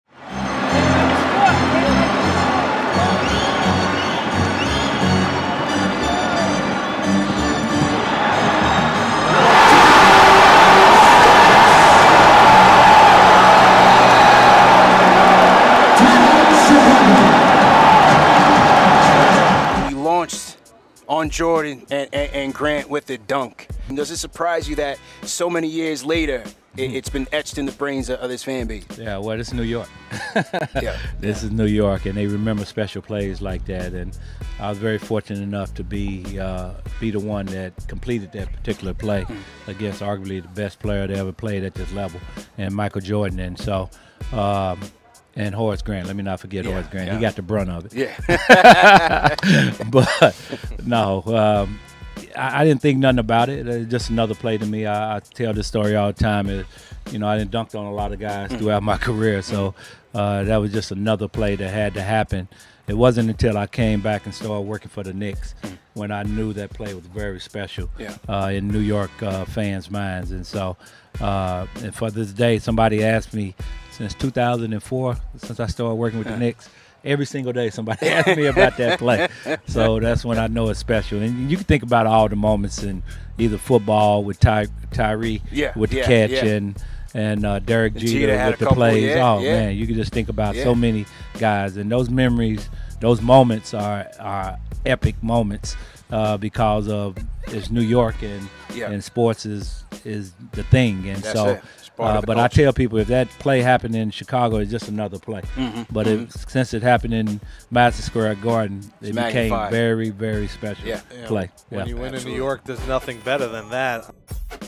New York Knicks Legend John Starks On What "The Dunk" Meant To Knicks Fans
Knicks legend John Starks joined Knicks Fan TV for an exclusive interview.  In this clip, Starks talks about his famous dunk on the Chicago Bulls and what it meant to Knicks fans.  Catch the full Starks interview in video or podcast format here:

🏀PODCAST LINKS! 🏀